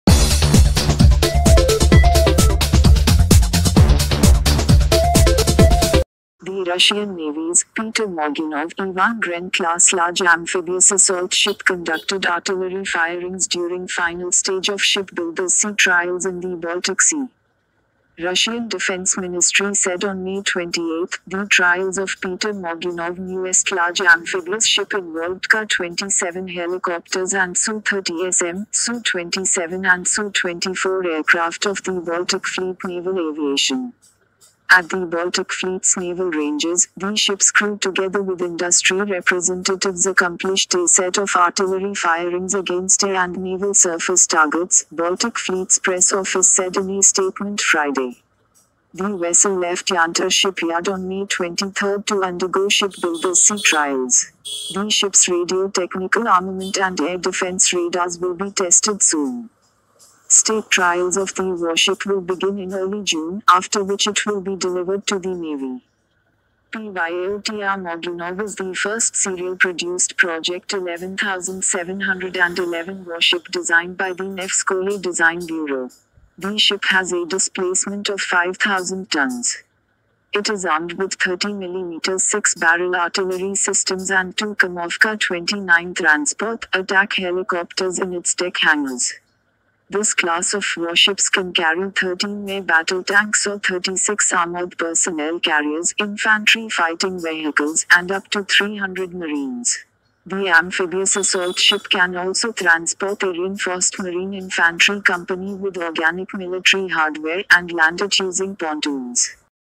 Russian Amphibious Assault Ship 'Peter Morgunov' conducts Artillery Trials
The Russian Navy’s ‘Peter Morgunov’ Ivan Gren-class large amphibious assault ship conducted artillery firings during final stage of shipbuilder’s sea trials in the Baltic Sea.